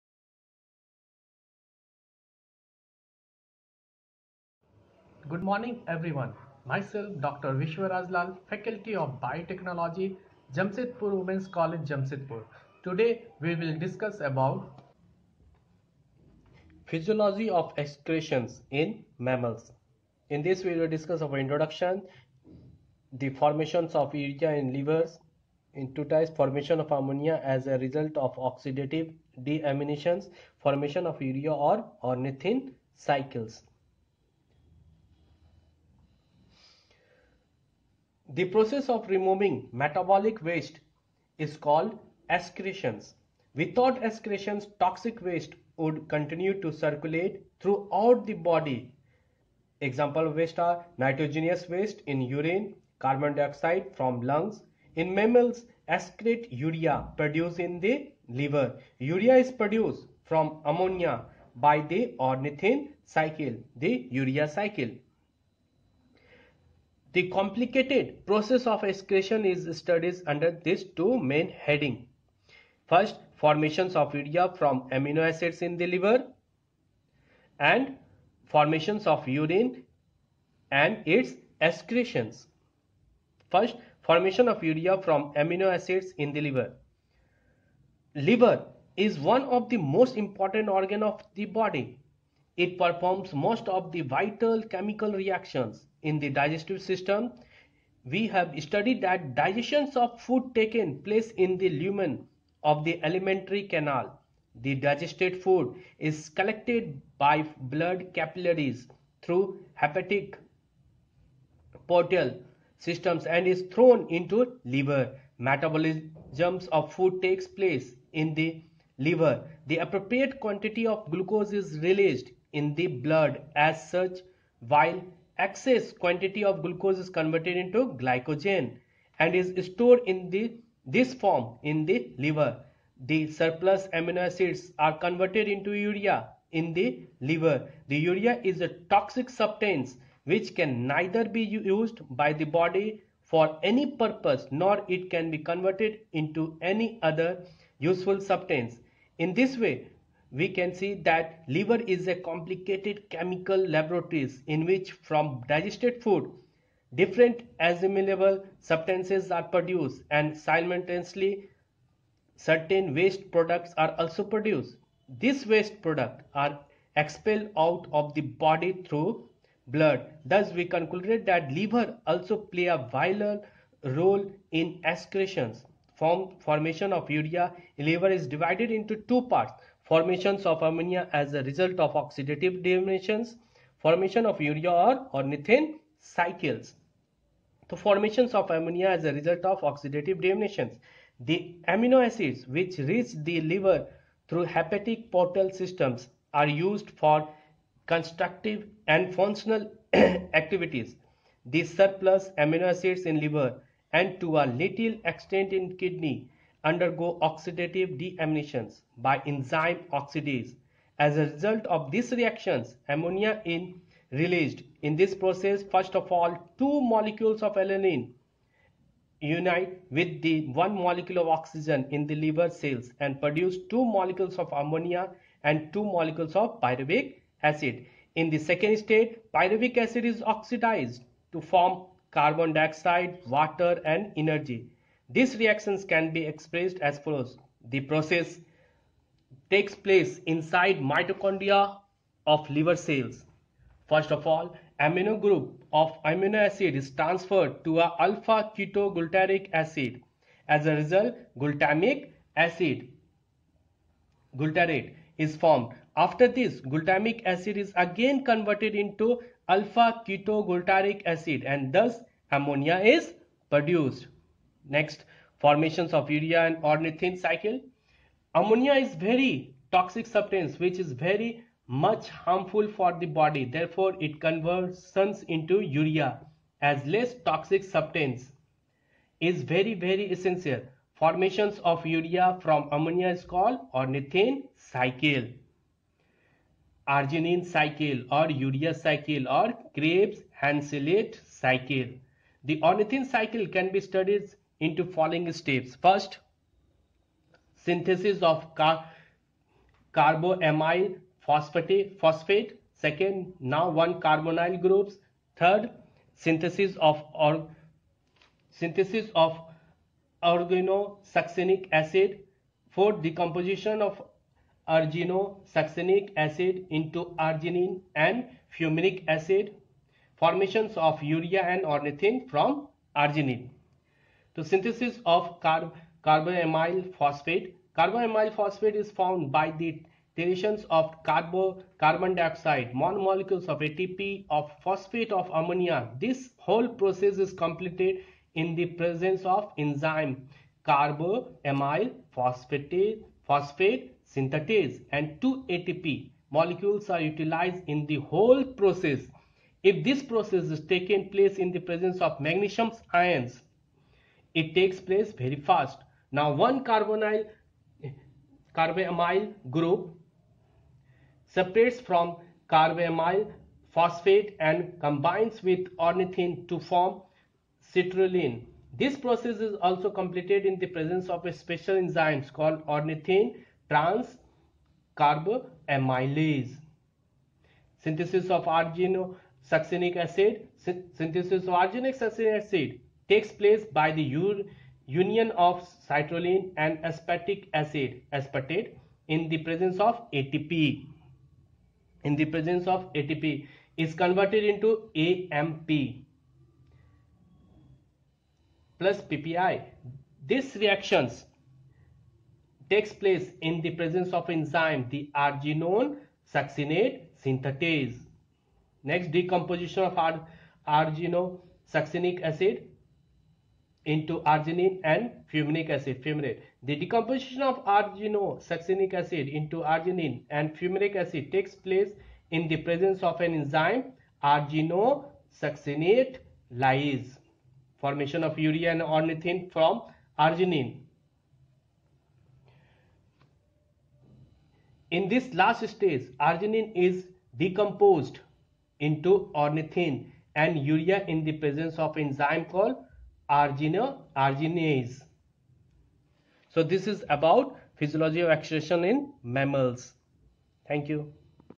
PHYSIOLOGY OF EXCRETION IN MAMMALS BY DR. VISHWA RAJ LAL , ASSISTANT PROFESSOR , BIOTECHNOLOGY
In mammals, excretion is the formation of urine in the kidneys, and expelling carbon dioxide from the lungs.

 These waste products are eliminated by urination and breathing out respectively.

If excretion does not occur in an organism, waste products accumulate, which eventually kill the organism.

Ornithine Cycle  (or urea-ornithine cycle), a cyclic enzymatic process consisting of consecutive transformations of the amino acid ornithine and leading to the synthesis of urea. The ornithine cycle is the most important means of assimilation of ammonia (and thus for its neutralization) in many species of animals, as well as in plants and microorganisms.

The ornithine cycle consists of three main reactions: the conversion of ornithine into citrulline, the conversion of citrulline into arginine, and the splitting of arginine into urea and ornithine . Reactions (I) and (II) require expenditures of energy, which is provided in the form of adenosine triphosphate (ATP). Reaction (I) proceeds in two steps: (1) the formation of carbamyl phosphate, which has an energy-rich phosphate bond, from ammonia, carbon dioxide, and two molecules of ATP (the reaction is activated by N-acetylglutamic acid; ammonia is apparently supplied to the liver in the form of glutamine, which is split by liver glutaminase into NH3 and glutamic acid); and (2) the formation of citrulline in the reaction of carbamyl phosphate with ornithine (the reaction proceeds using the energy of the carbamyl phosphate bond). Reaction (II) also has two stages: (1) the reaction of citrulline with aspartic acid to form arginosuccinic acid (the reaction proceeds with the participation of ATP, with the liberation of adenylic acid [adenosine monophosphate, or AMP] and pyrophosphate, H4P2O7); and (2) the splitting of arginosuccinic acid to yield arginine and fumaric acid. In reaction (III), arginine is hydrolyzed to urea and ornithine, which reenters the cycle.